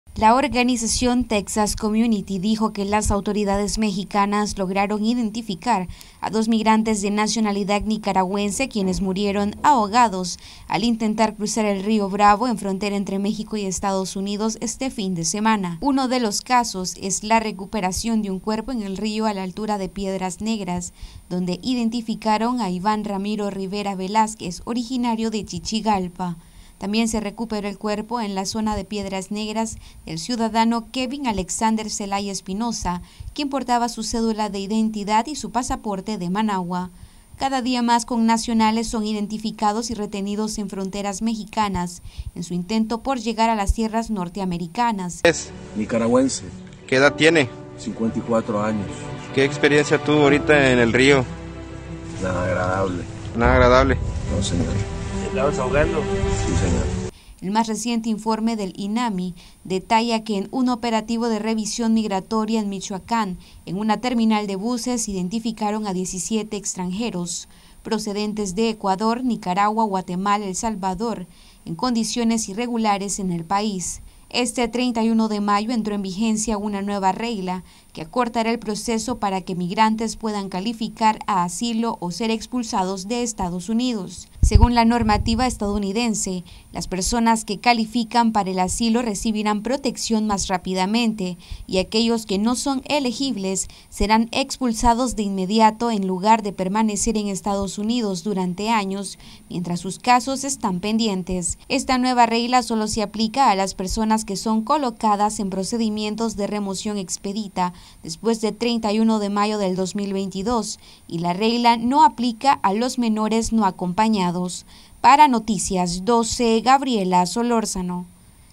Dos migrantes nicaragüenses mueren ahogados al intentar cruzar el río Bravo
Noticias12Nicaragua Dos migrantes nicaragüenses mueren ahogados al intentar cruzar el rio bravo en frontera entre México y EE.UU, este fin de semana. Además recientemente las autoridades de Norteamérica aprobaron una nueva regla que acortará el proceso para que migrantes puedan calificar a asilo o ser expulsados